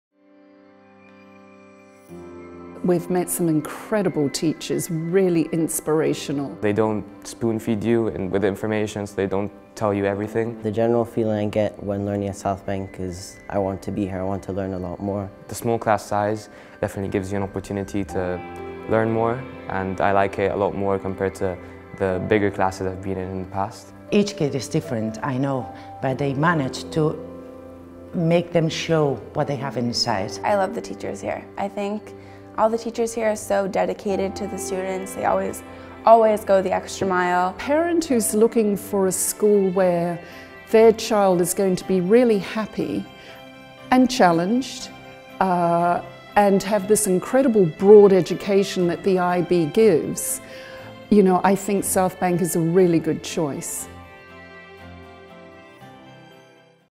About the teachers at Southbank International School
Southbank students and their parents talk about the teaching at Southbank International School